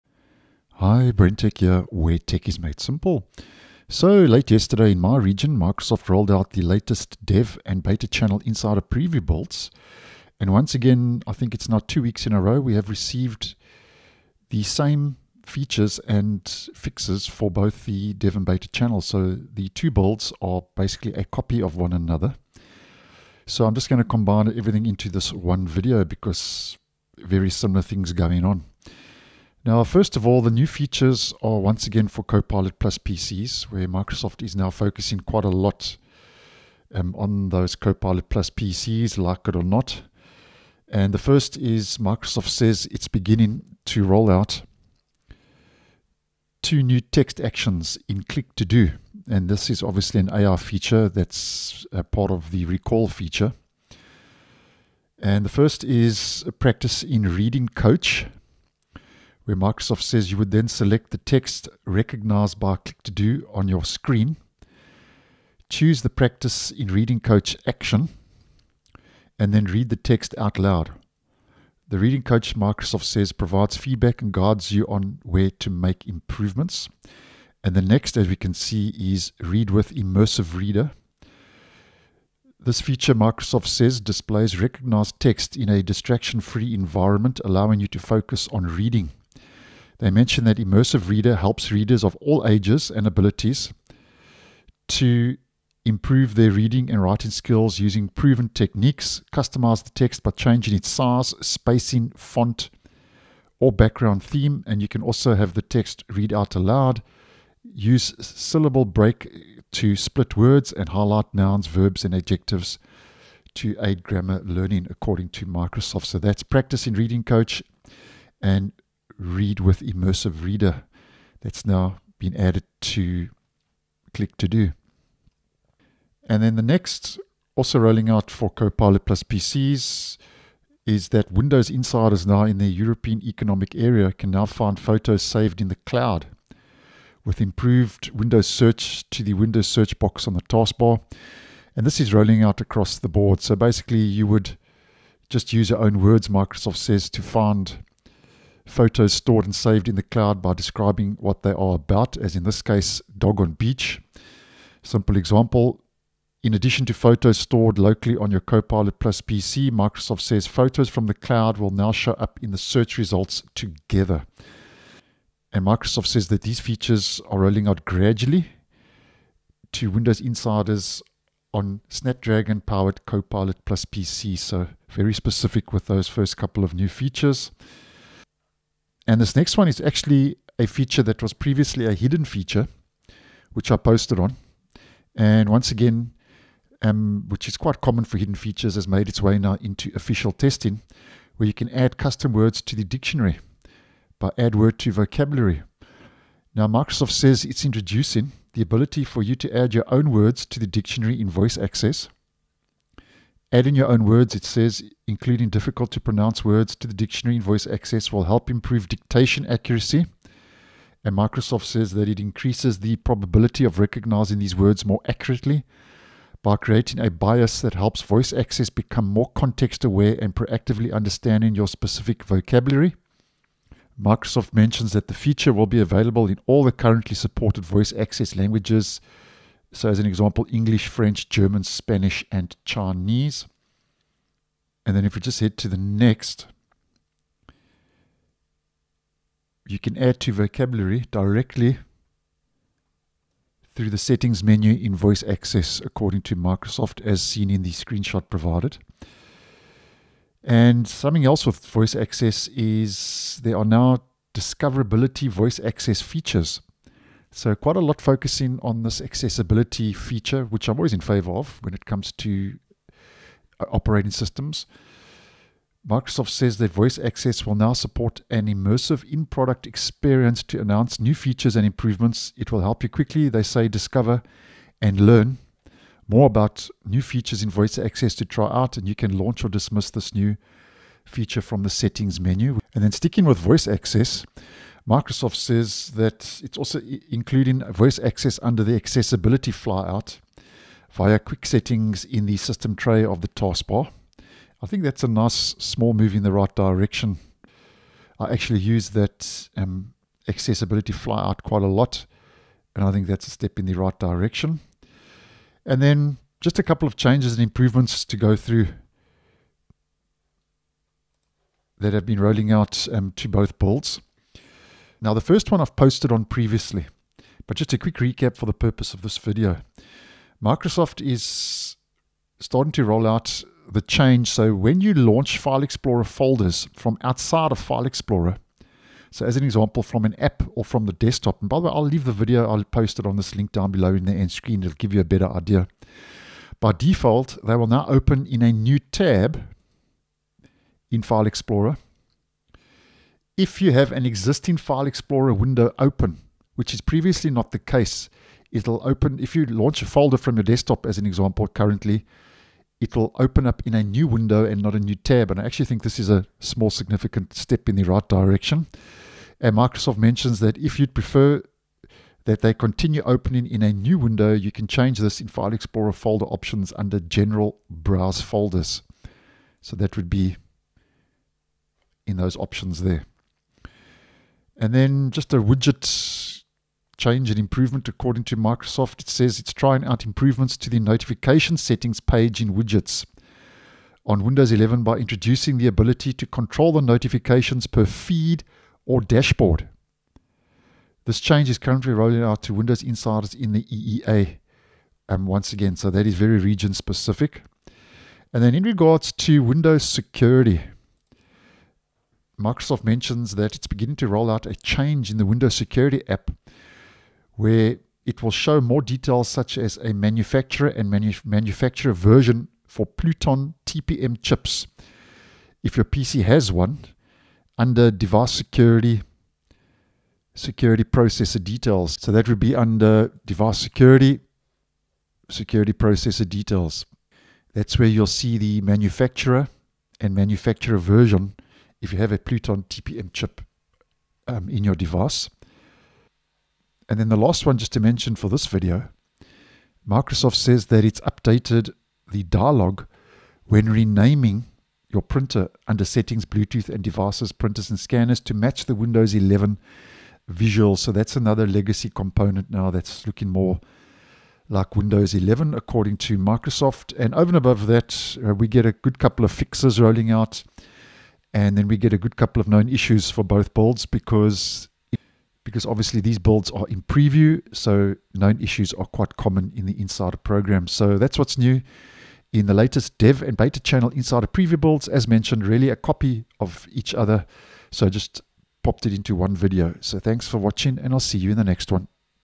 What's New in Windows 11 Builds 26120.3872 and 26200.5562?
These latest builds for the Dev and Beta channel insiders with updates KB5055640 and KB5055642, include new Copilot+ PC features, improved Windows Search, enhancements to Voice Access and more.
Windows 11 Will Finally Let You Open Folders in New Tabs (What's New in Build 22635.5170)